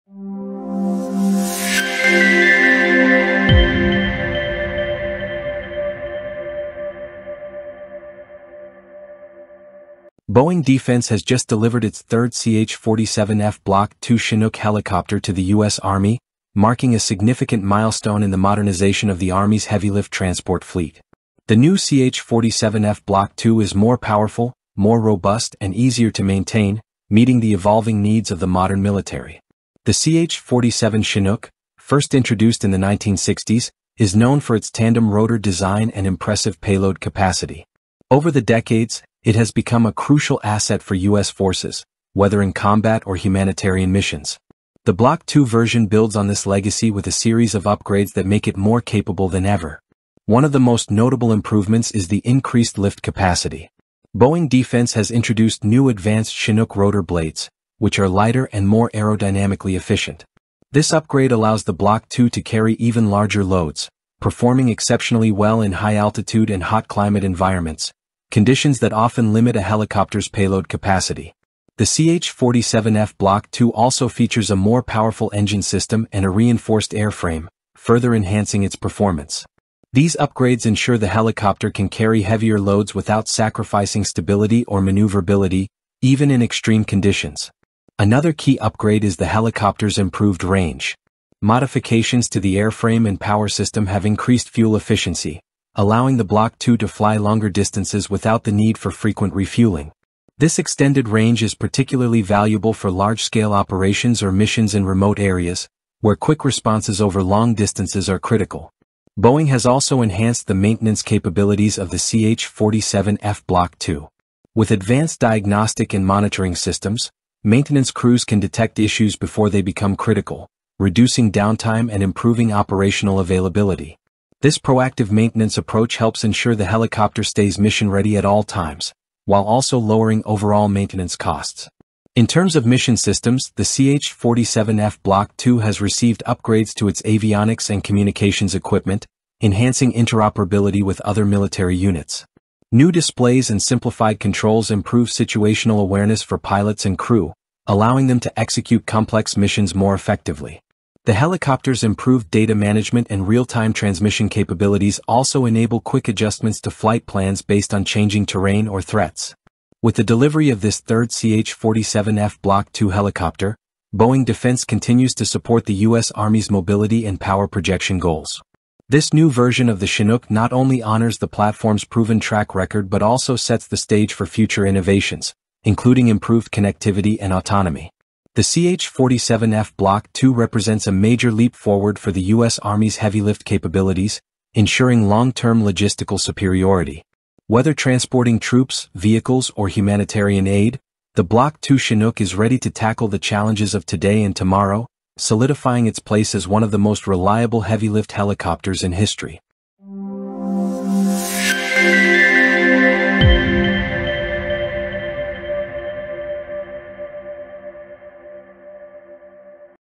CH 47F Block II A New Era of Power and Flexibility for the U S Army's Heavy Lift Helicopters
Boeing Defense recently delivered the third CH-47F Block II Chinook helicopter to the U.S. Army, advancing the modernization of its heavy-lift transport fleet. The CH-47F Block II features upgraded rotor blades, a reinforced airframe, and a more efficient power system, increasing payload capacity and operational range. These improvements make it ideal for high-altitude and hot climate conditions, essential for long-distance missions. Enhanced maintenance systems, better avionics, and advanced communication technologies further boost the Chinook's performance, ensuring high operational availability. This helicopter continues to evolve, solidifying its role in U.S. Army operations and future logistical superiority.